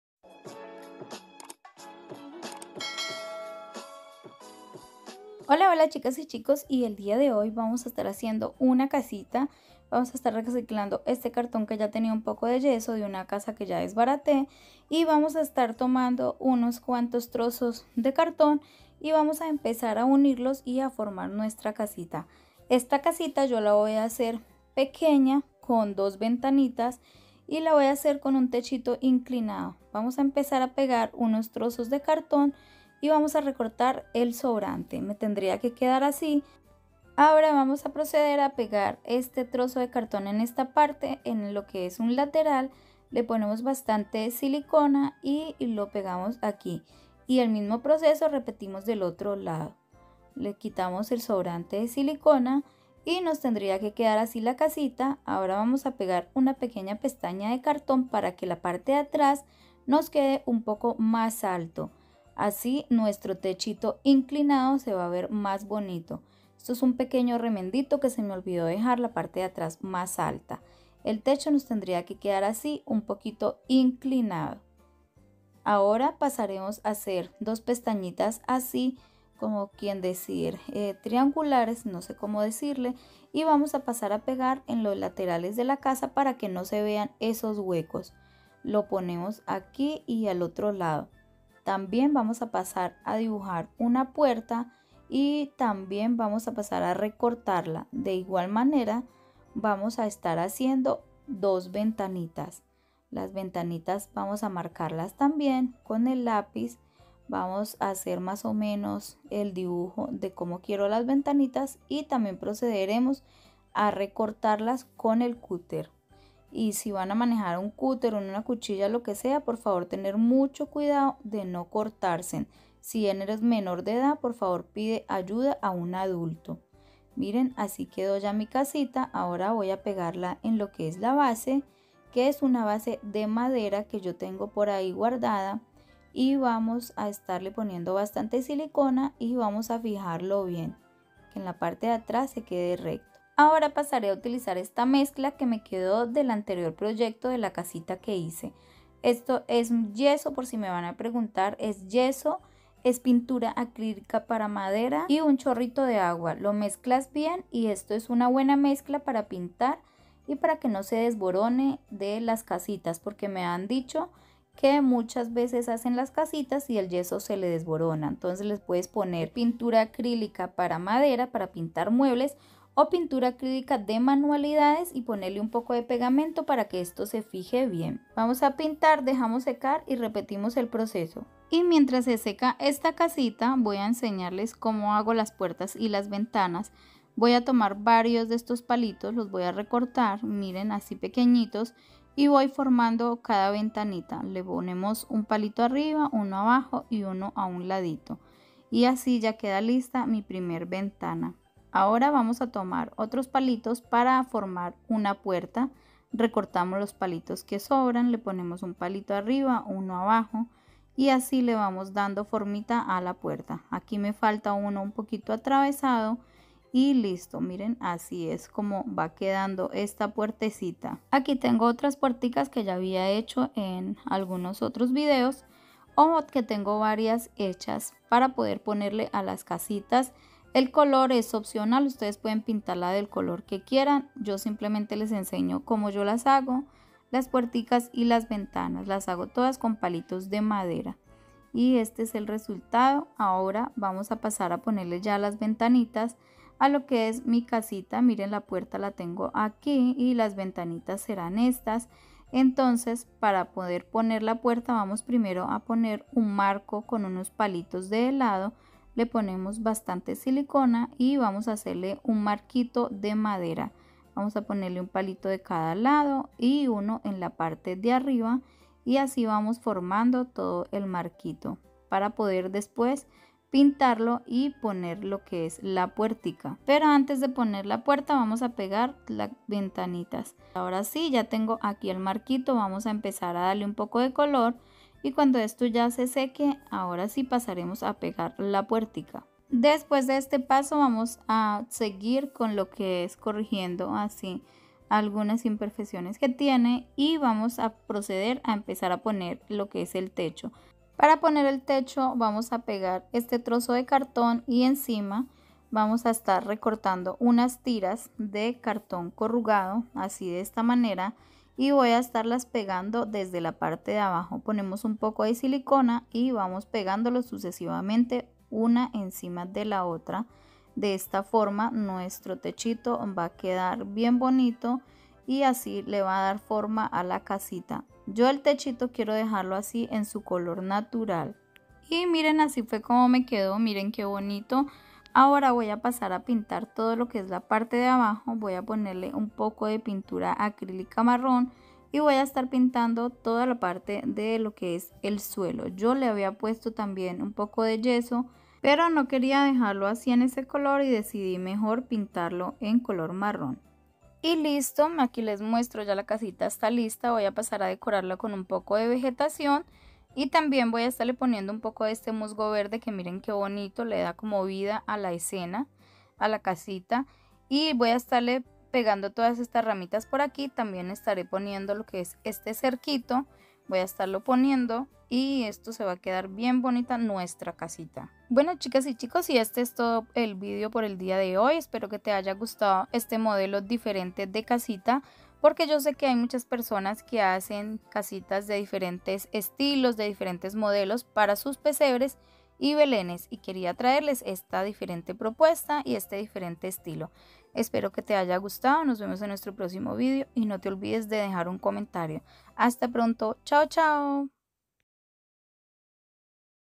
CASITA PARA EL PESEBRE FACIL Y RAPIDO 🛖 CASA PARA BELEN (casita 4)
hola chicas y chicos , hoy queria compartir esta casita # 4 con ustedes , estare subiendo varias ideas de casitas para el belen , espero les guste 😊👍🌠